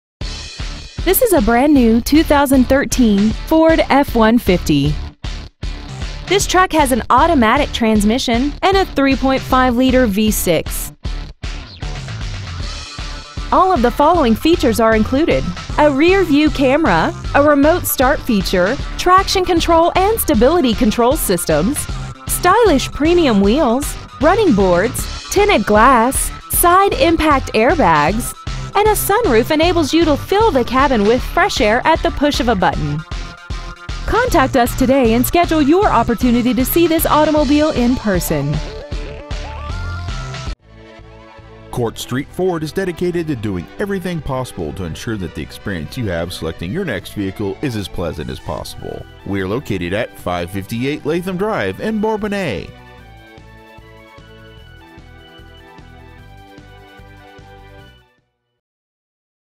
2013 FORD F-150 Bourbonnais IL
Year : 2013
 Make : FORD
 Model : F-150
 Engine : 3.5L V6 24V GDI DOHC TWIN TURBO
 Trans . : 6-SPEED AUTOMATIC
 Exterior : SILVER
 Miles : 62
 Interior : BLACK
 Stock : TR13301